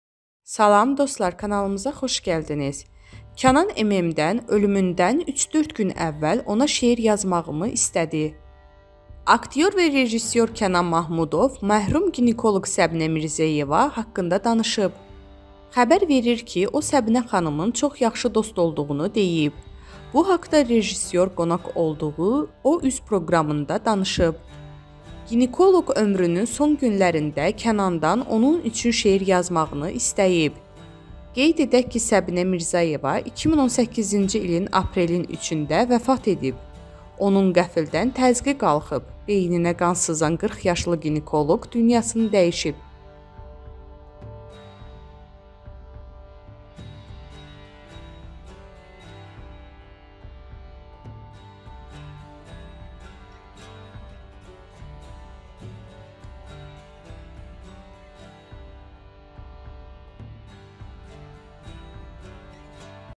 Kənan M.M: "Ölümündən 3-4 gün əvvəl ona şeir yazmağımı istədi"
Ölümündən 3-4 gün əvvəl ona şeir yazmağımı istədi!